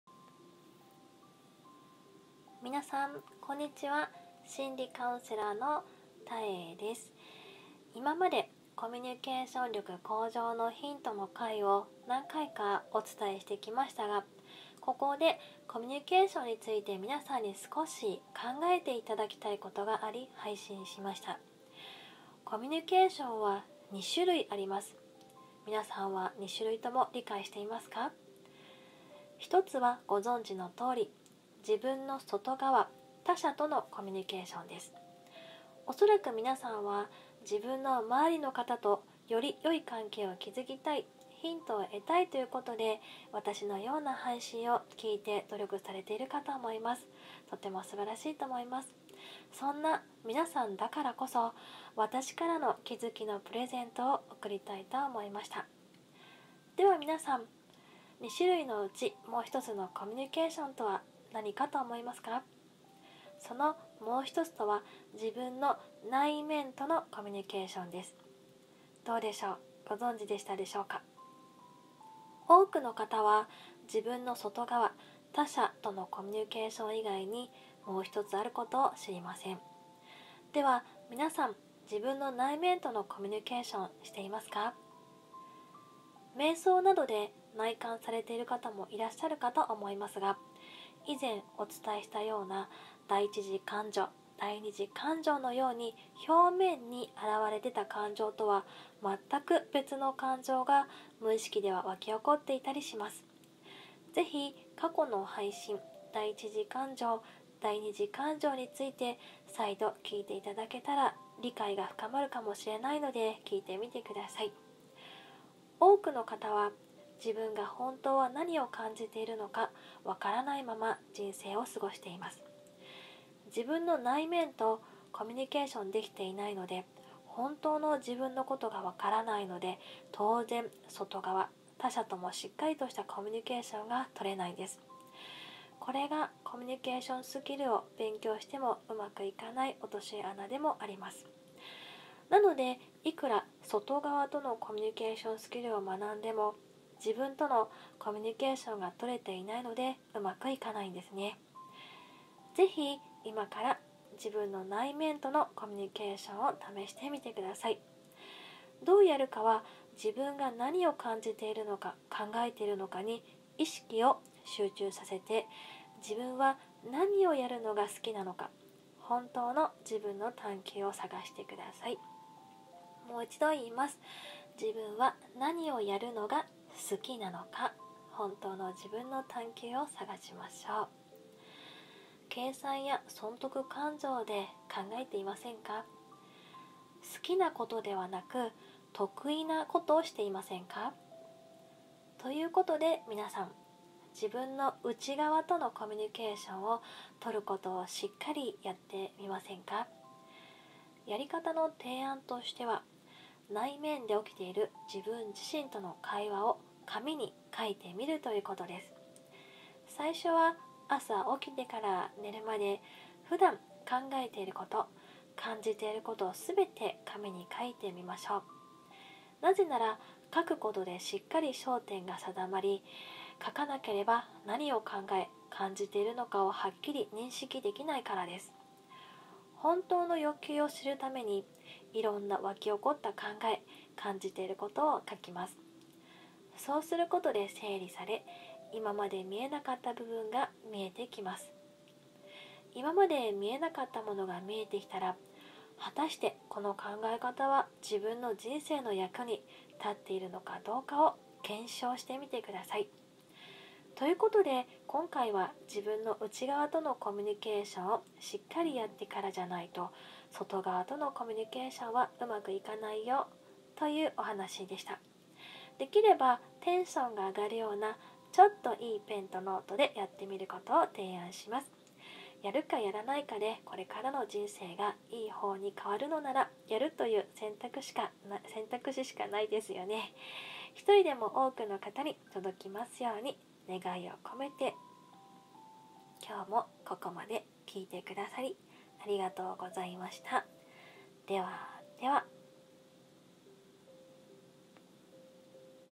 「本当の自分探求」相手と素敵な関係を作るコミュニケーション力向上のヒント⑦
たえラジオより
YouTubeにあげてみました。

自分の内側とのコミュニケーションがしっかりできてなければ外側(他者)とのコミュニケーションがうまくいかないことに氣づいて頂けたらとおもいます。

自分は何をやるのが好きですか？
得意な事ではなくて好きな事をしていますか？
本当の自分を探求してみましょう。

コミュニケーション力向上のヒント② 第一次感情と第二次感情